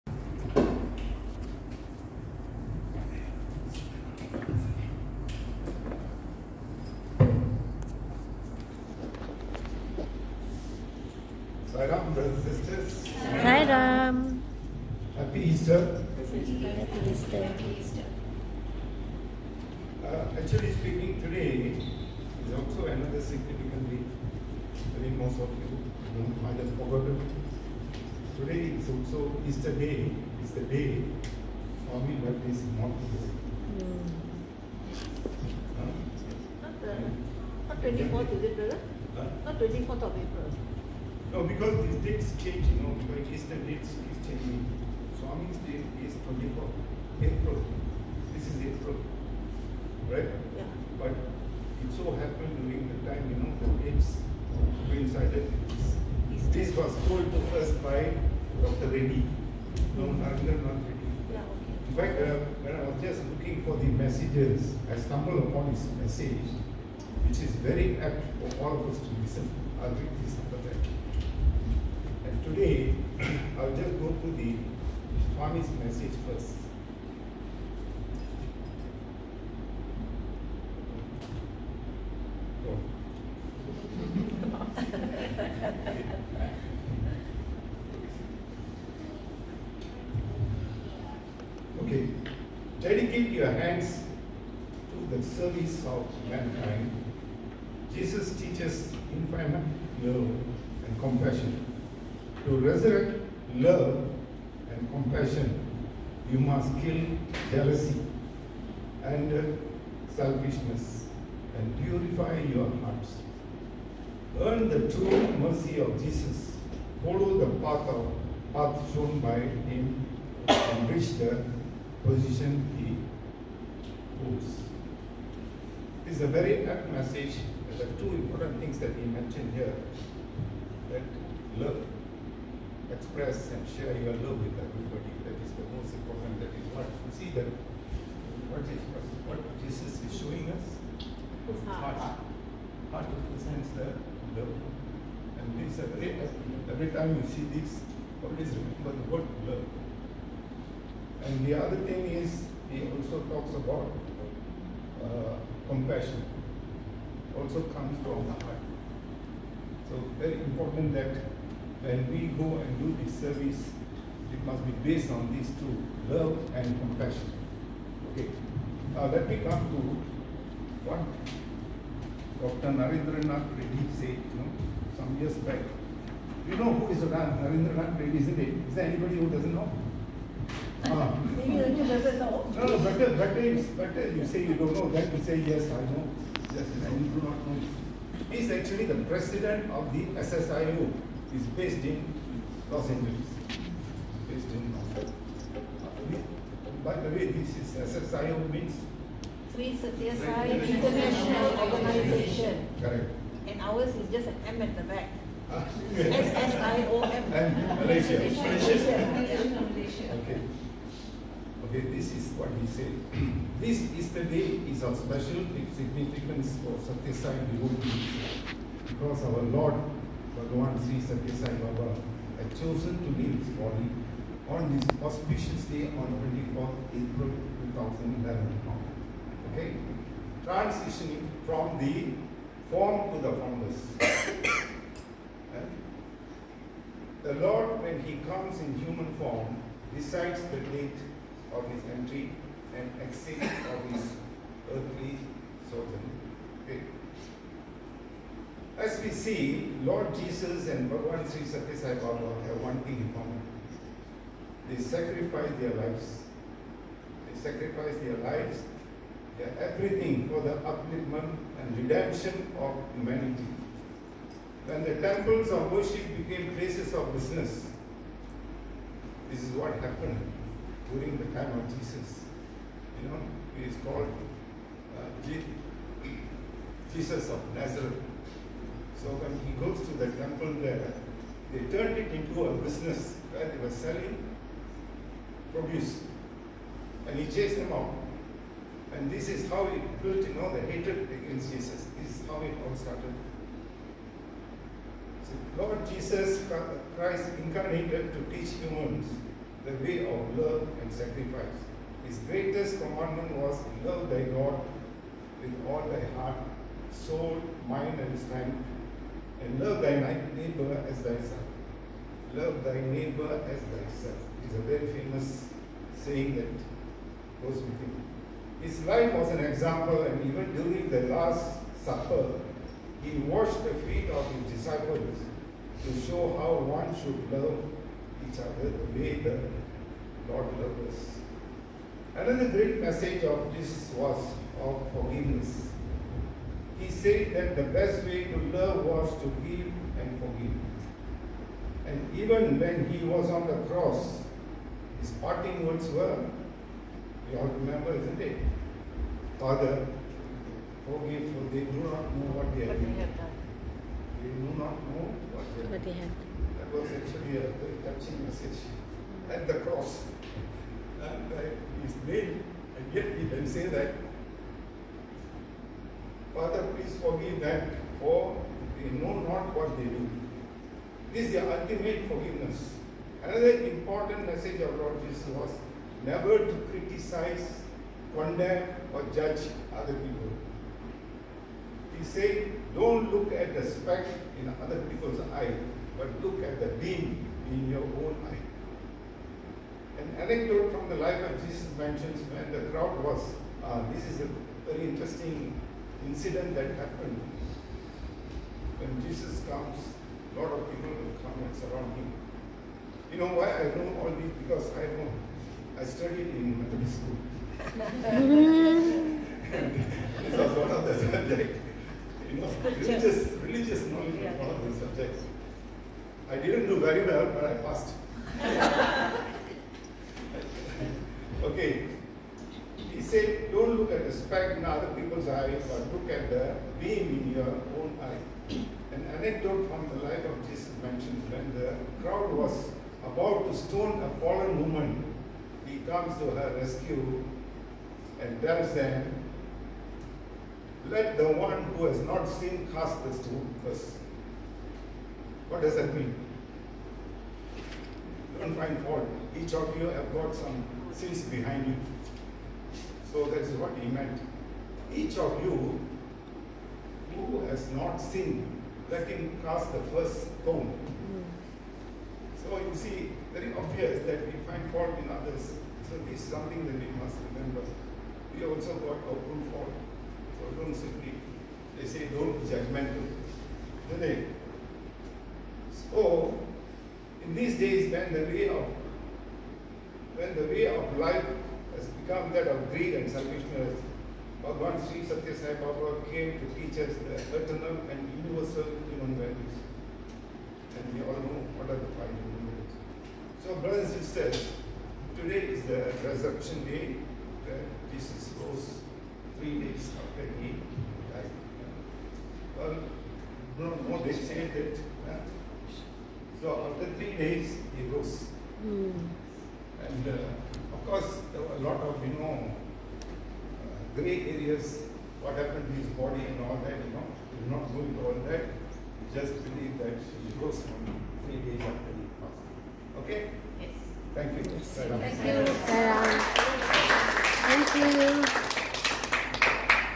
Bhagawan Sri Sathya Sai Baba's Easter Sunday Message delivered by Bro Rajoo
Sathya Sai Baba Message on Christ.
Dedicate your hands to the service of mankind. Jesus teaches infinite love and compassion, you must kill jealousy and selfishness and purify your hearts. Earn the true mercy of Jesus. Follow the path shown by him and reach the position he holds.